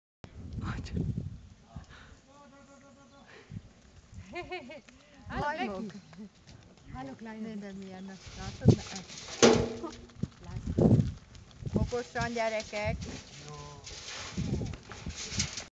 feeding American Bison/ Amerikai Bölény etetés
2 year old heifers, they're very tame :)